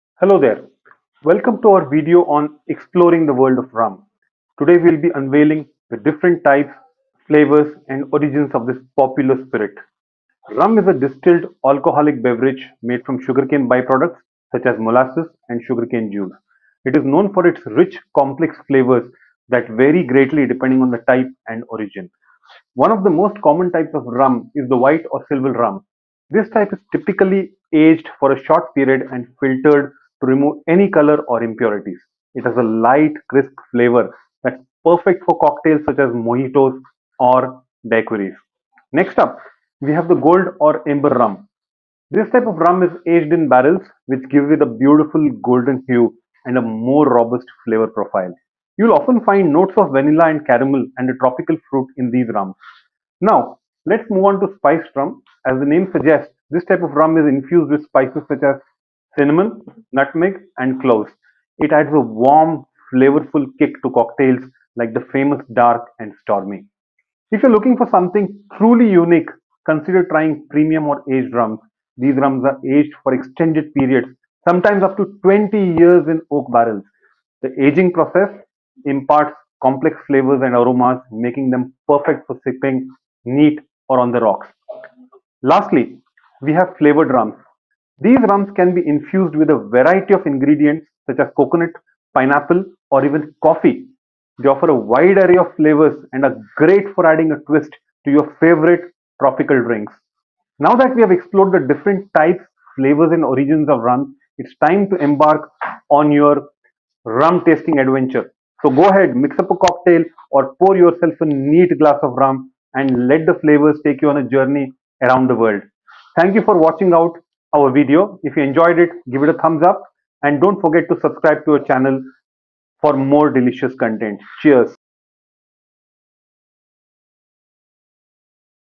Exploring the World of Rum
Welcome to our video, "Exploring the World of Rum: Unveiling Different Types, Flavors & Origins"! In this comprehensive guide, we dive deep into the fascinating realm of rum, tackling the various types, flavors, and mesmerizing origins that make this spirit truly unique.

Get ready to embark on a tasty journey as we uncover the secrets behind rum's diverse flavor profiles and learn about the rich history behind each variation. Whether you're a rum enthusiast or simply curious about expanding your beverage knowledge, this video is for you!

Join us as we explore the classic types of rum, including light rum, dark rum, gold rum, and spiced rum. Discover what sets these variations apart, from their distillation processes to the ingredients used. We'll delve into the production techniques employed by renowned rum distilleries worldwide, shedding light on the unique craftsmanship that goes into every bottle.

Prepare your taste buds for an adventure as we take a closer look at the astounding flavors that rum has to offer. From the smooth and delicate notes found in white rum to the robust and caramel-like undertones of aged dark rum, we'll uncover the vast spectrum of flavors awaiting you. Get ready to learn how to distinguish between different rum flavors and find your personal favorite!

We can't forget the origins that have shaped the world of rum! From the Caribbean islands to Latin America, each region has its own distinct rum traditions and production methods. Discover the fascinating stories and cultural influences behind internationally acclaimed rum-producing countries, and gain a deeper understanding of the diverse geographies that contribute to their unique rums.

So, whether you enjoy sipping on a refreshing mojito, delighting in a classic daiquiri, or simply appreciate the earthy flavors of a neat rum pour, this video is here to enhance your rum knowledge. Join us now and expand your understanding of the splendid world of rum!

Don't miss out on this captivating exploration of "Exploring the World of Rum: Unveiling Different Types, Flavors & Origins." Cheers to the beauty of rum! 🥃